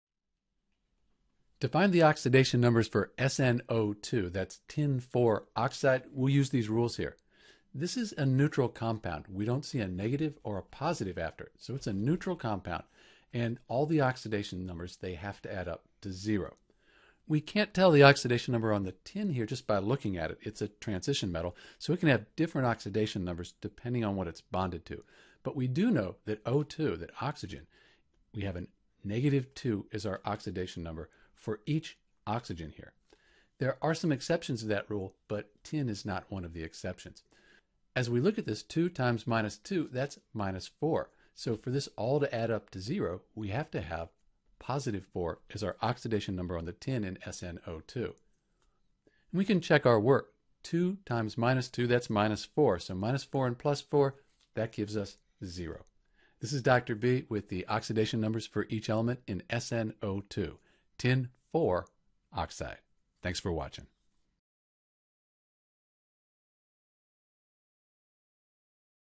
How to find the Oxidation Number for Sn in SnO2
To find the correct oxidation state of Sn in SnO2 (Tin (IV) oxide), and each element in the compound, we use a few rules and some simple math.

First, since the SnO2 doesn’t have an overall charge (like NO3- or H3O+) we could say that the total of the oxidation numbers for SnO2 will be zero since it is a neutral compound.

We write the oxidation number (O.N.) for elements that we know and use these to figure out oxidation number for Sn.

Here it is bonded to element symbol so the oxidation number on Oxygen is -2. Using this information we can figure out the oxidation number for the element Sn in SnO2.